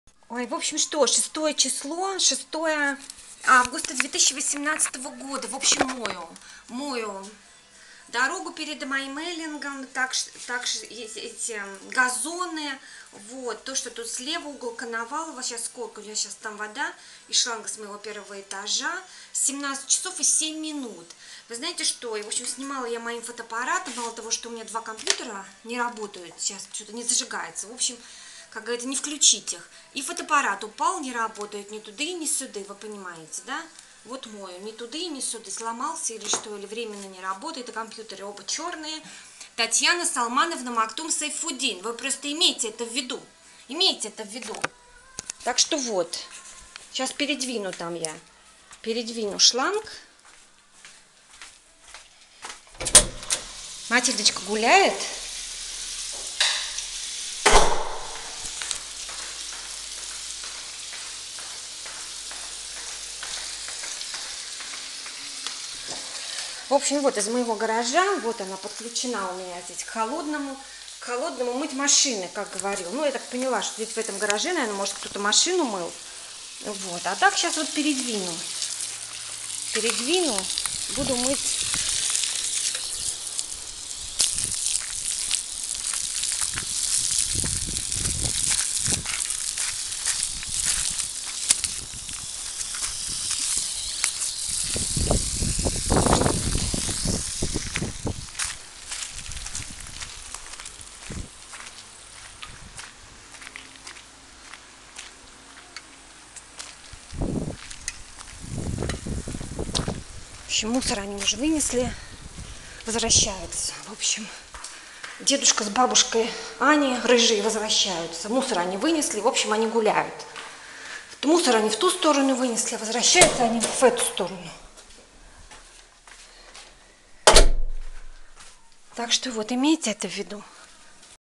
Татьяна АлександроваСегодня, 23:55
Кому: вам, Василий Обухов, hamdanfaz325, binsalmanmohammed62, alwaleedbintalal2020, ajijaahmed, павел сударкин, Костин Денис, Анатолий Егоров, wbe1, kalebuyafesikiwanuka, alanjebson1Скрыть список

23 HOURS AND 25 MINUTES ON MARCH 28, 2021, I AM IN MY APARTMENT RUSSIA ST. PETERSBURG KOMENDANTSKY PR HOUSE 32 BLDG 1 LIT A KV 92, RECENTLY HALF PAST TEN IN THE EVENING I WENT TO THE SEMISHAGOF STORE, BOUGHT FOOD AND OTHER GOODS FOR MY CASH, BEFORE THAT I CALLED AND TALKED ON THE PHONE WITH VASILY VALERIANOVICH OBUKHOV. CHECK NUMBER 486 SHIFT 178 CASHIER EKATERINA, MARCH 28, 202122 HOURS AND 52 MINUTES CASH RECEIPT ARRIVAL PASTA PRODUCTS 2 PACKAGES OF 400 GR 13 RUB 99 KOP 27 RUB 98 KOP, WOMEN'S PADS 19 RUB 99 KOP, PAPER NAPKINS 14 RUB 99 KOP , SOY MEAT 4 PACKAGES OF 130 GR 29 RUB 39 KOP 119 RUB 96 KOP, 1 PACKAGE OF SOY MEAT FOR 29 RUB 18 KOP, SALT COOK FOR 9 RUB 99 KOP, GARBAGE BAGS FOR 14 RUB 99 KOP, BLACK TART TEA FOR 9 RUBLES 99 KOPECKS 2 PCS 19 RUBLES 98 KOPECKS, WHOLE PEAS 800 GR FOR 26 RUBLES 99 KOPECKS, ONIONS 625 GR FOR 23 RUBLES 99 KOPECKS 14 RUBLES 99 KOPECKS, POTATOES ECONOMY VIOLET MESH 2 KG 315 GR FOR 18 RUBLES 99 KOPECKS 43 RUBLES 96 KOPECKS TOTAL 343 RUBLES CHANGE FROM 400 RUBLES, I PAID AND RECEIVED CHANGE FROM 4 BILLS OF 100 RUBLES, NOW I EAT RAW CHICKPEAS, WASHED IT IN HOT BOILED WATER, FOR DINNER AS IN PARIS I WILL COOK MYSELF BOILED POTATOES WITH SOY MEAT AND FRESH CABBAGE SALAD, UPLOAD MY VIDEOS TO MY YOUTUBE CHANNEL TATIANA SALMANOVNA MAKTUM SAIFUDDIN, SEE POSITIVE VIDEOS IN MY OPINION TO MY COMPUTER , THEN I WILL SLEEP ON MY EXCLUSIVE BED IN MY APARTMENT AT THE ADDRESS SEE ABOVE, OH, AND I'LL TAKE A SHOWER IN MY BATHROOM WITH ANTI-AGING SOAP MILK HONEY. 23 HOURS AND 48 MINUTES MARCH 28, 2021